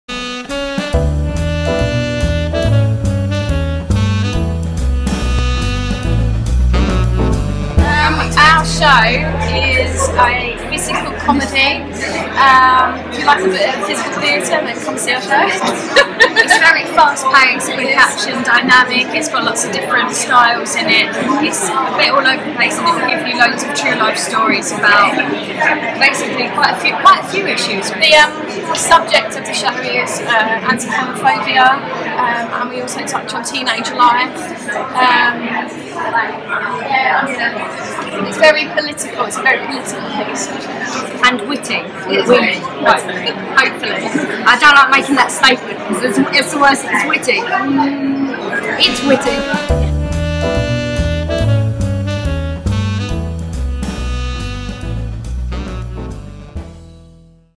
Before It Starts at Camden People's Theatre 4-7 August
Growing up is hard and when a new girl starts school and reveals her controversial sexuality her friends have a lot to laugh about. But when the jokes turn sour and the playful banter becomes bullying, the group realise they have a lot to be sorry for. Friendships are put to the test in this edgy, brash and full frontal take on sex, social media and school life.
This latest production by Naked Frank Theatre tackles the issue of homophobia in a witty and unapologetic nature. Inspired by the true stories of young people that have personally been bullied.
An eclectic mix of comedy, physical performance and urban characters; BEFORE IT STARTS is a bold and hard-hitting take on teenage life through the eyes of a seventeen year old.